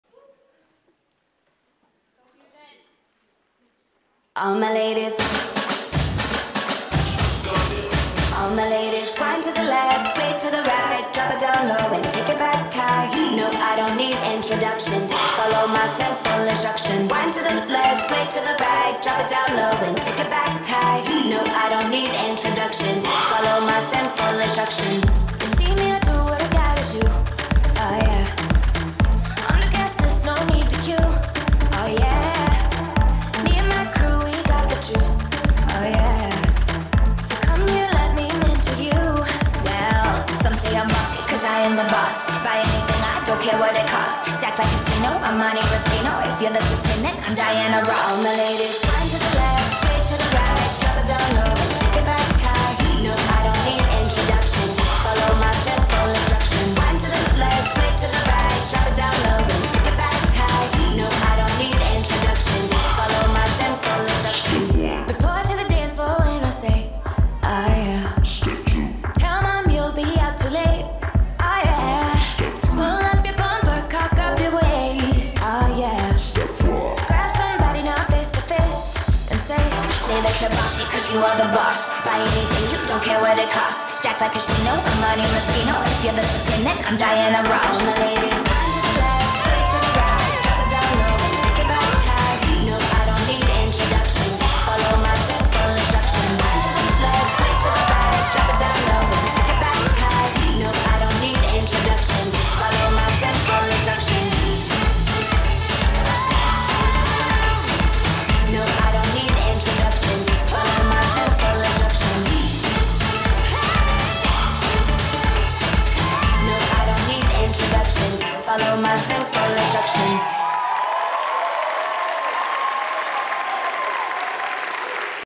Instruction FUDE 2022
Fairfield University Dance Fusion Team at Fairfield University Dance Ensemble show - April 3, 2022